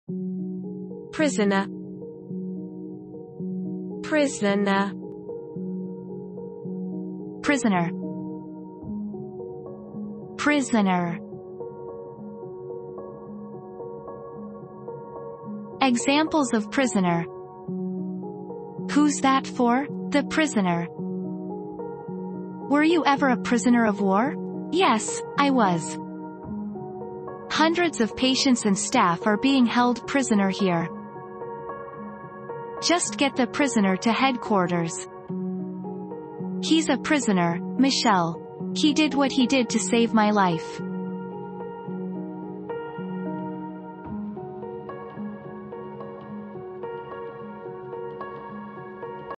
PRISONER pronunciation • How to pronounce PRISONER
How to say Prisoner in British English and American English?